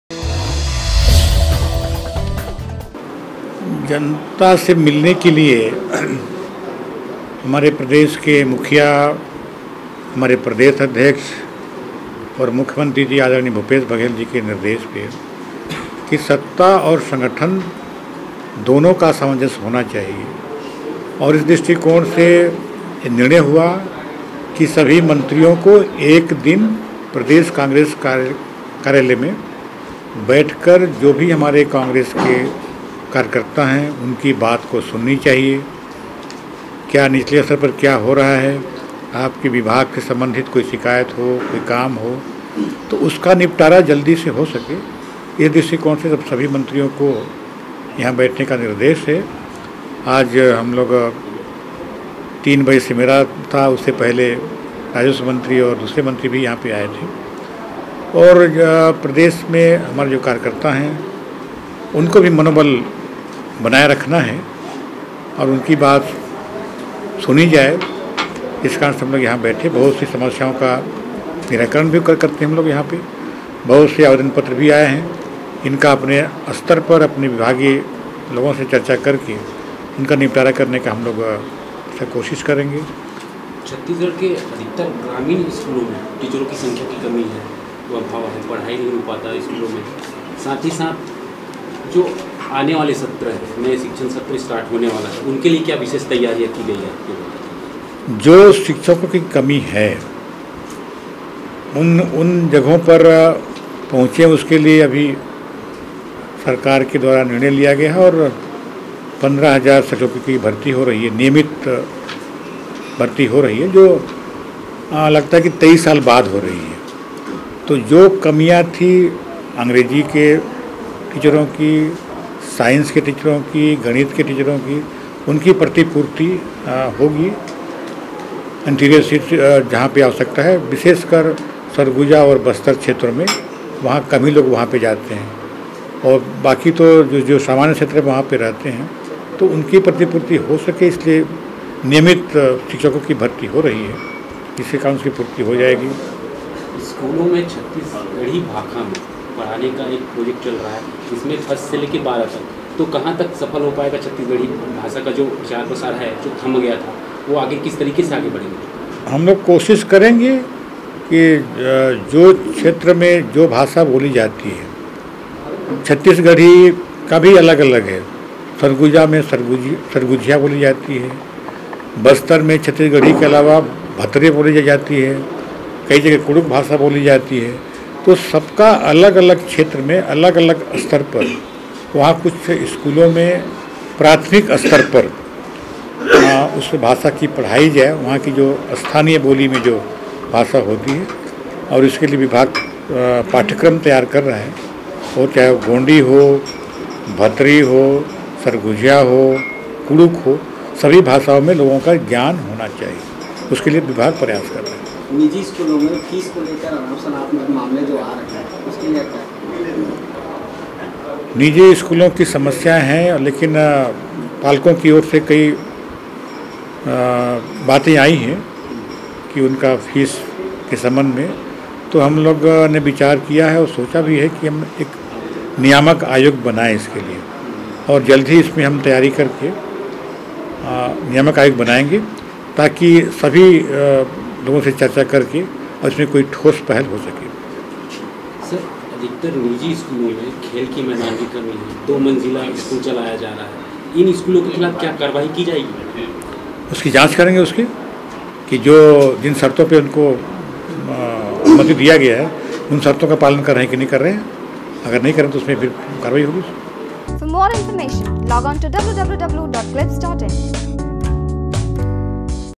Private school में वसूल रहे मनमानी फीस करेंगे चर्चा, Premsai Singh Tekam
आज Rajiv Bhawan में जनता व कार्यकर्ताओं से रूबरू होने स्कूल Minister of Education Premsai Singh Tekam पहुंचे थे। इस दौरान उन्होंने बताया कि निजी स्कूलों में वसूल रहे मनमाने Fees को लेकर नियामक आयोग बनाकर चर्चा की जाएगी।
स्कूल शिक्षा मंत्री Premsai Singh Tekam ने पत्रकारों से चर्चा करते हुए कहा कि जनता से मिलने के लिए प्रदेश की मुखिया, प्रदेश अध्यक्ष व मुख्यमंत्री के निर्देश पर सत्ता और समर्थन दोनों का समावेश होना चाहिए। इसी दृष्टिकोण से निर्णय हुआ कि सभी मंत्रियों को एक दिन बैठकर जनता की बात सूने और उनकी समस्याओं का निपटारा जल्द हो। जिससे आज 3 बजे से मेरा जवाबदारी था। इसके अलावा प्रदेश में कार्यकर्ता है उनका भी मनोबल बनाए रखना है। आज बहुत से आवेदन पत्र आए है उसमें अपने विभाग से चर्चा कर जल्द ही निपटाने की कोशिश किए है। वहीं ग्रामीण स्तरों में शिक्षकों की कमी को लेकर उन्होंने कहा कि जहां पर शिक्षकों की कमी है उसके लिए सरकार के द्वारा निर्णय लिया गया है। 15 हजार शिक्षकों की भर्ती हो रही है। जिसमें अधिकांश अंग्रेजी व विज्ञान के शिक्षकों की पूर्ति होगी। वहीं बस्तर और सरगुजा में शिक्षकों की अधिक कमी है, जिससे वहां के क्षेत्र में शिक्षकों की भर्ती की जा रही है। वहीं स्कूलों में छत्तीसगढ़ी भाषा की सफल होने की बात पर उन्होंने कहा कि अलग-अलग क्षेत्र में अलग-अलग तरह की छत्तीसगढ़ी भाषा बोली जाती है। जिससे उसे उस क्षेत्र की स्थानीय बोली पर पढ़ाया जाए उसके लिए निर्णय लिया गया है। इसके अलावा निजी स्कूलों में अनाप शनाप फीस लेने की बात पर मंत्री प्रेमसाय ने कहा कि इस तरह की समस्या पालकों की ओर से आई है। उसके लिए नियामक आयोग बनाकर जल्द ही चर्चा की जाएगी। वहीं निजी स्कूलों को जिन शर्तो पर मान्यता दी गई है उन शर्तो की पालन कर रहे है या नहीं इसकी जांच कर उस पर कार्रवाई की जाएगी।